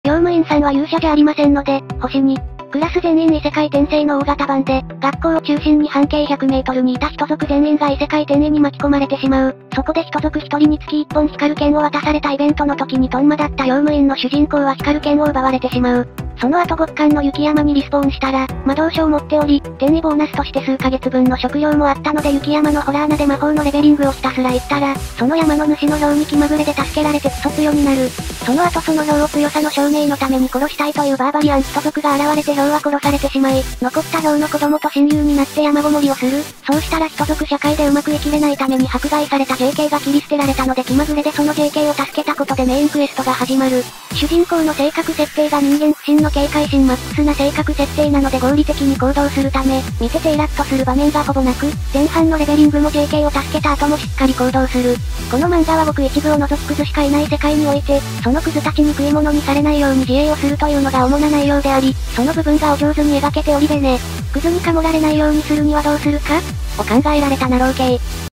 【★★・】用務員さんは勇者じゃありませんので / 長田馨 棚花尋平【あらすじ 漫画レビュー 映画レビュー】
用務員さんは勇者じゃありませんので、☆２

暮らす全員異世界転生の大型版で、
學校を中心に半径１００メートルにいた
人族全員が異世界転移に巻き込まれて
しまう、そこで人族一人につき一本光る剣を
渡されたイベントの時にとんまだった用務員
の主人公は光る剣を奪われてしまう

そのあと極寒の雪山にリスポーンしたら、
魔導書を持っており、転移ボーナスとして
数か月分の食料もあったので雪山の洞穴で
魔法のレベリングをひたすら行ったら、
その山の主のヒョウに気まぐれで助けられ
てクソつよになる

そのあとそのヒョウを強さの証明のために
頃したいというバーバリアン人族が現れて
ヒョウは頃されてしまい、残ったヒョウの
子供と親友になって山籠もりをする、そう
したら人族社会でうまく生きれない為に
迫害されたＪＫが切り捨てられたので気ま
ぐれでそのJKを助けた事でメインクエスト
が始まる

主人公の性格設定が人間不信の警戒心マックス
な性格設定なので合理的に行動するため、みて
てイラっとする場面がほぼ無く、前半のレベリ
ングもＪＫを助けた後もしっかり行動する

この漫画はごく一部を除き屑しかいない世界に
おいて、その屑たちに食い物にされないように
自営をするというのが主な内容であり、その部分
がお上手に描けておりベネ

屑にカモられないようにするにはどうするか？
を考えられたなろう系

Let's play !! - ゲーム実況